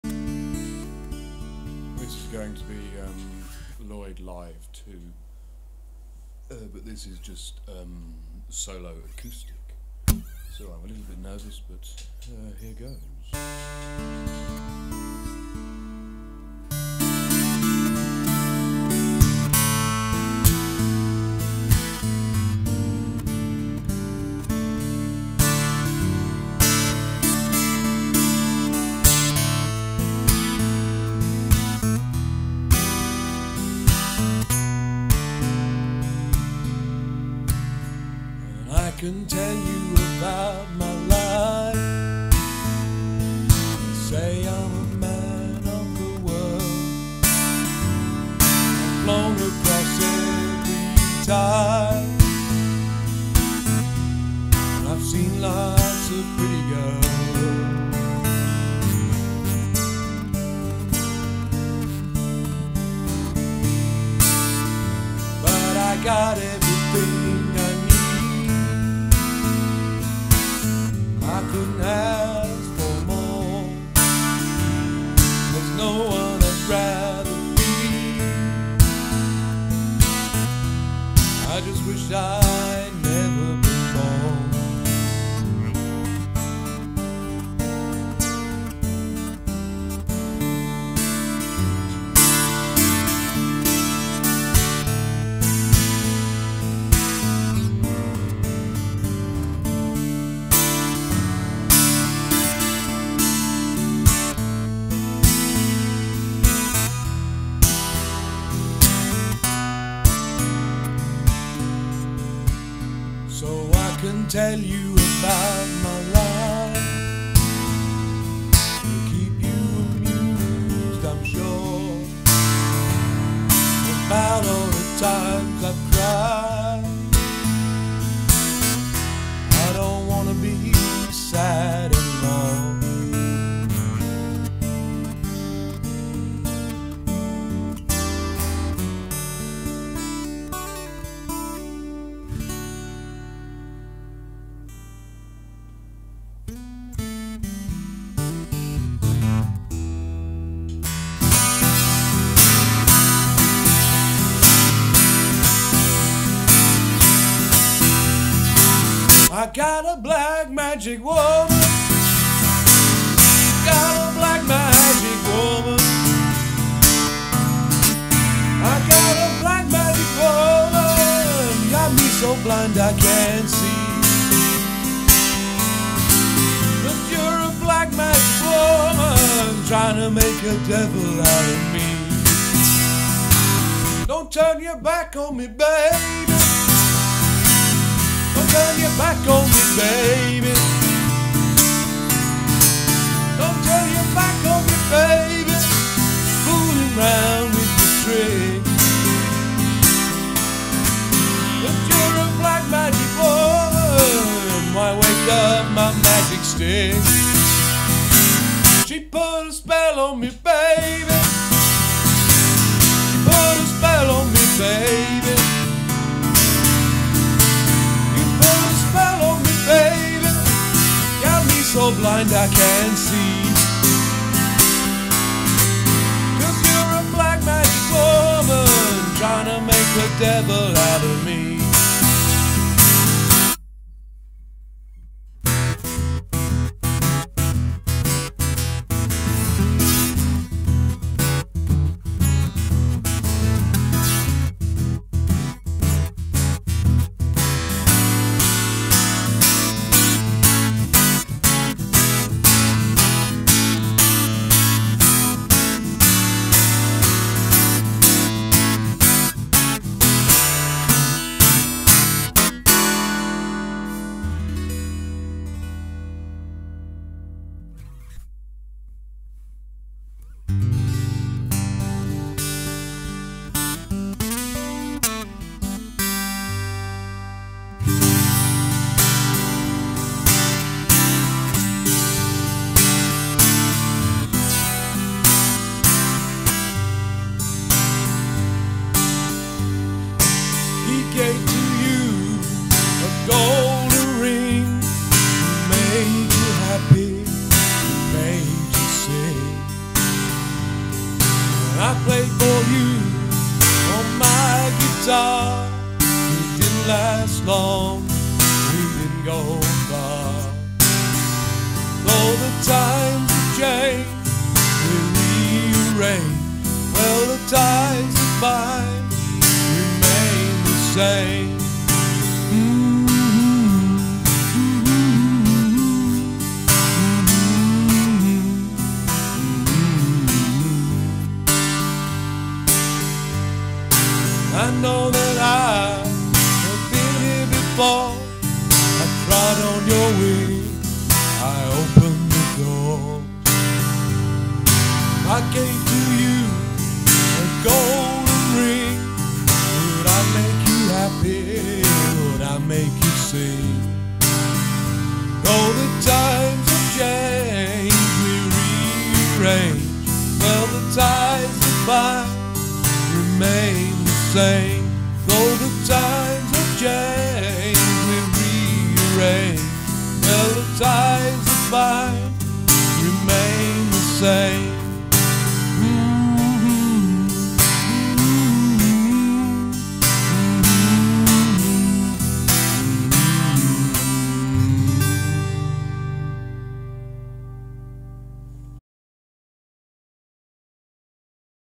LLOYD LIVE 2 (Fleetwood Mac and EC Covers)
This is my second Lloyd Live featuring; Fleetwood Mac and Eric Clapton